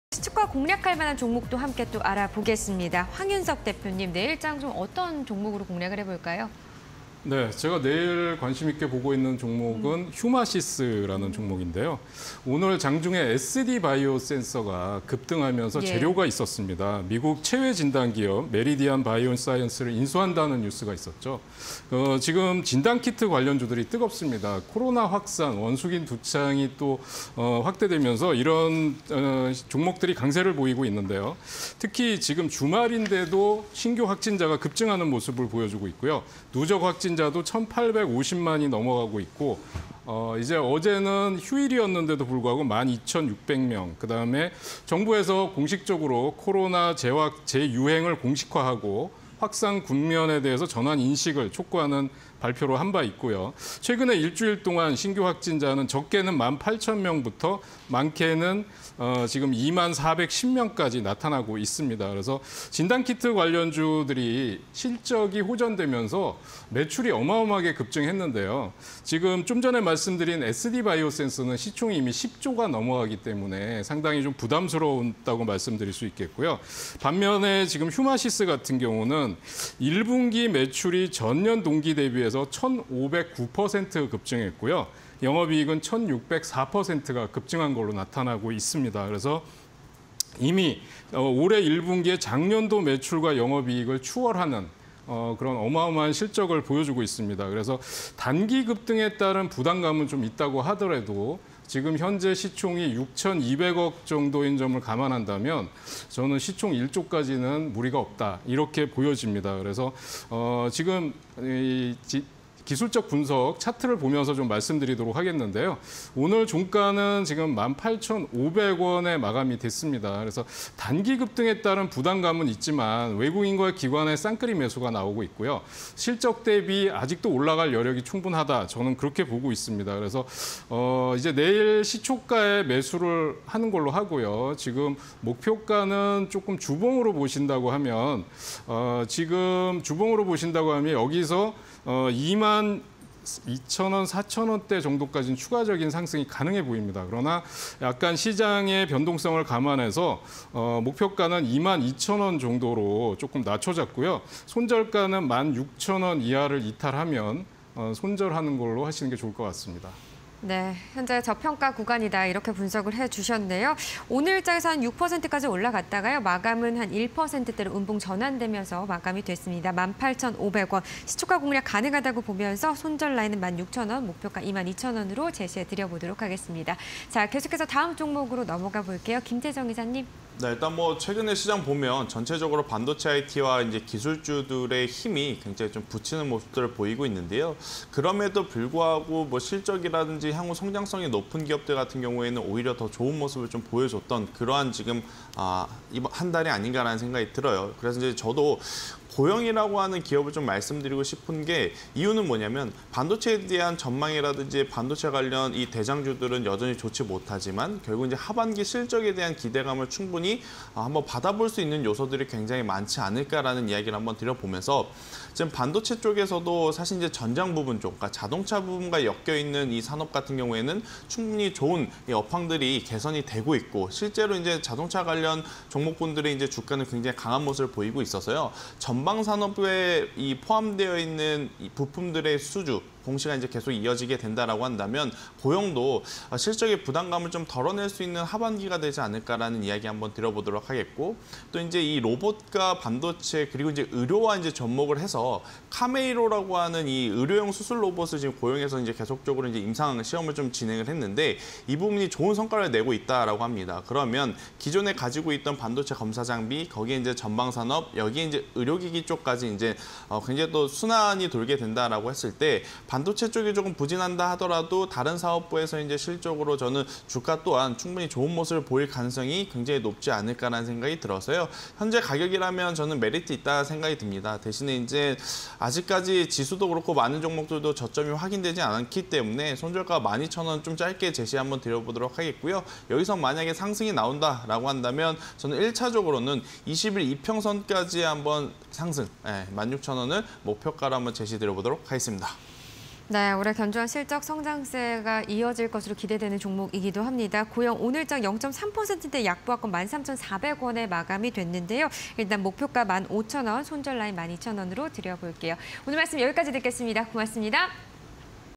휴마시스(205470), 고영(098460)_사가지 (20220711)
휴마시스(205470)
- 최근 코로나 확진자 가파른 증가세
- 진단키트 관련주, 1분기 매출 급증
- 단기 급등에 따른 부담 있으나 저평가 판단

고영(098460)
- 시장, 기술주 성장동력에 주목
- 로봇ㆍ디지털기술ㆍ의료, 접목
- 의료로봇 '카이메로' 성과 본격화